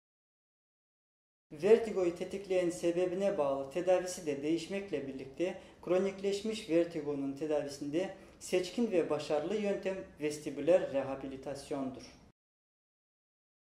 Vertigo tedavisi nasıl yapılır? - Op. Dr. Dastan Temirbekov
Taksim KBB ekibi 'Vertigo tedavisi nasıl yapılır?'' konusu hakkında bilgi veriyor.